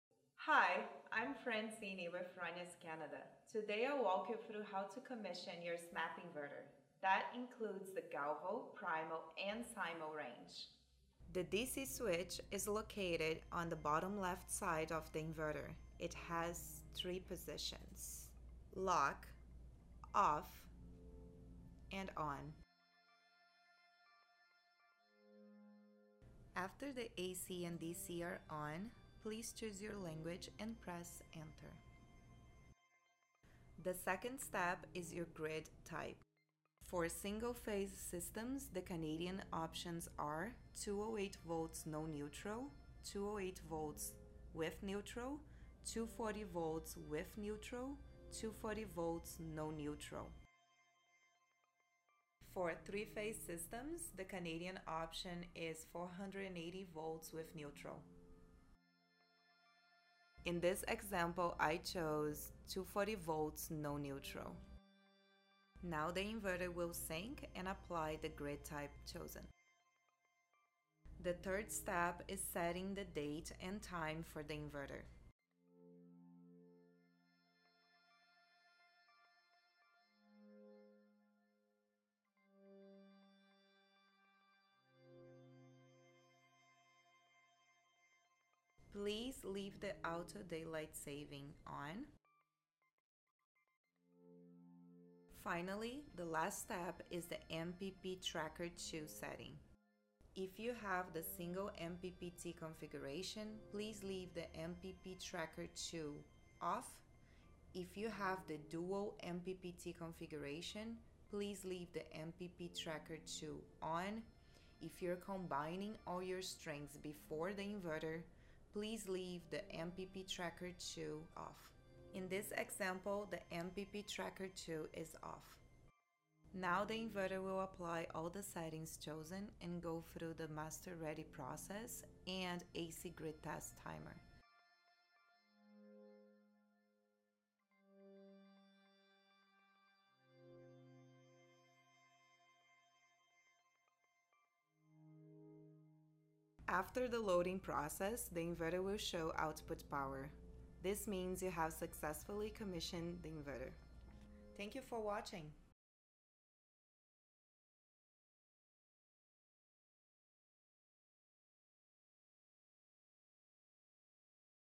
SnapINverter Comissioning Walkthrough (CA)
In this video, we are walking you through how to commission your SnapINverter. The settings used are specific to Canada.

Inverter Commissioning Checklist:
1. Turn on AC & DC
2. Language
3. Select Country - Grid Type
4. Date
5. Time
6. Auto Daylight Savings
7. MPP Tracker 2 - Single Vs. Dual MPPT
8. Grid Test Timer